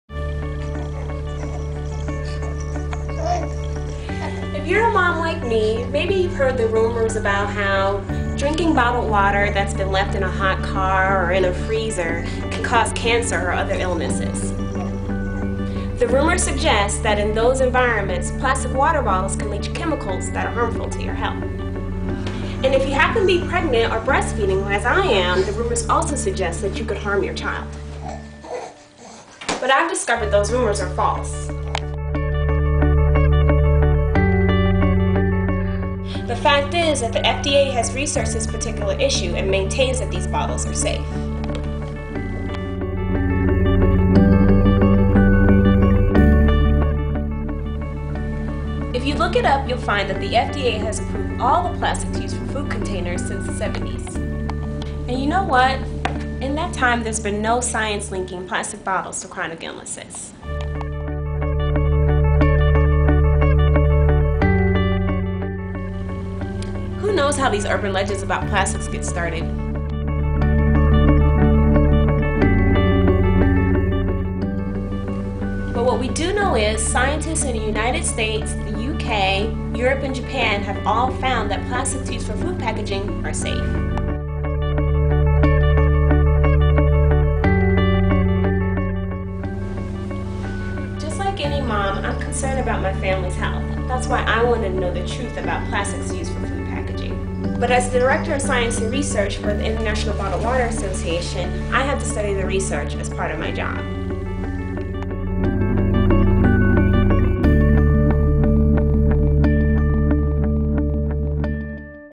Bottled Water Safety: Hot cars and frozen bottles
A Myth Buster: This video looks at the concern about drinking bottled water that has been left in a hot car or in the freezer. Conclusion: no links to cancer or other illnesses.